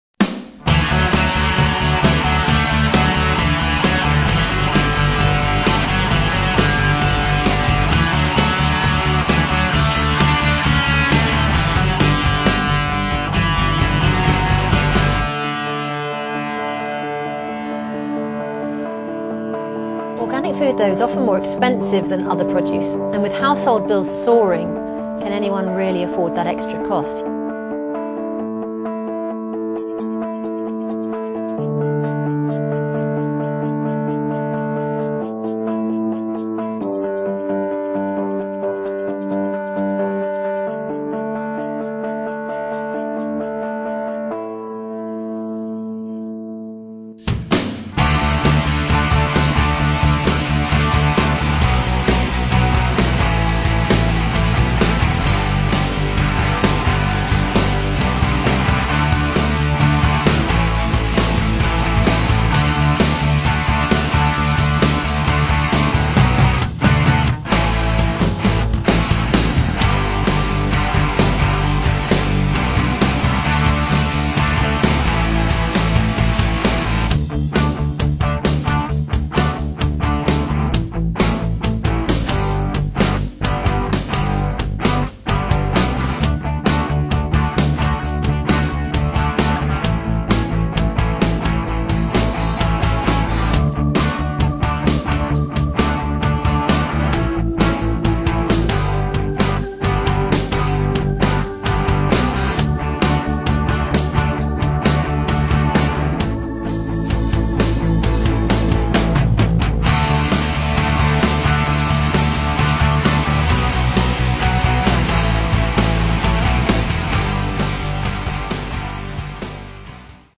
Why I Love Organic - Haygarth
Haygarth is working with over 90 companies from the organic industry to launch a three year 'Why I Love Organic' campaign designed to democratise organic. The integrated campaign encompasses national press advertising, PR and digital marketing and centres around a core platform of the many reasons to love organic, be it better animal welfare, better for nature, natural and great tasting food.